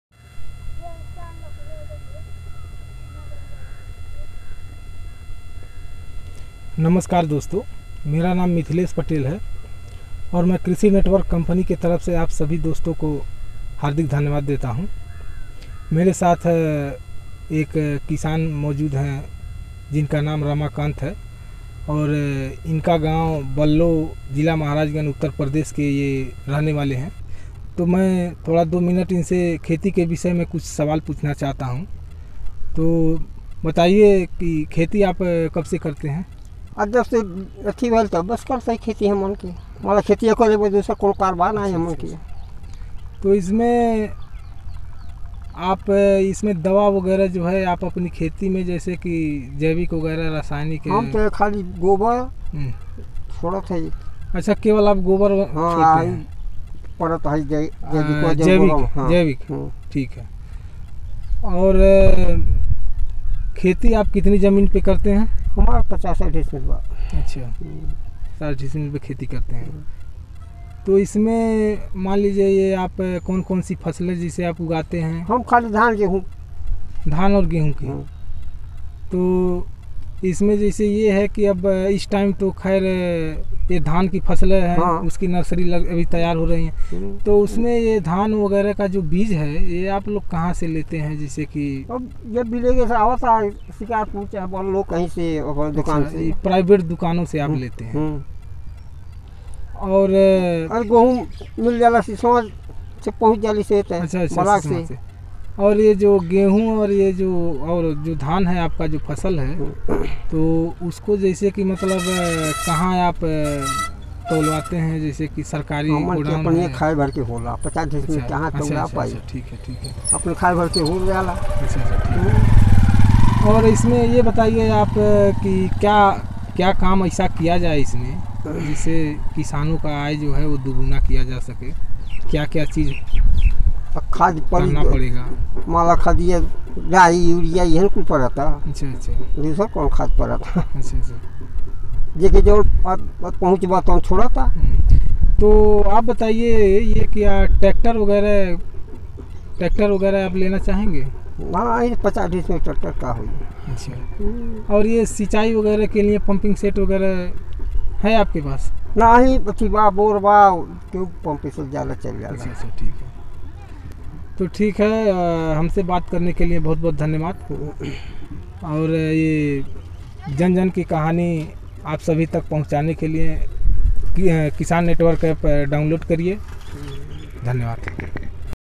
ग्राम प्रचारक मिथिलेश पटेल जिला महराजगंज के किसान रमाकांत जी से कृषि की चर्चा करते हुए
कृषि नेटवर्क हर ग्राम पंचायत में ग्राम प्रचारक बना रहा है | ये सभी ग्राम प्रचारक कृषि नेटवर्क व किसान के बीच एक कड़ी की तरह काम करते है | ग्राम प्रचारक अपने क्षेत्र में कृषि नेटवर्क का प्रतिनिधि होता है |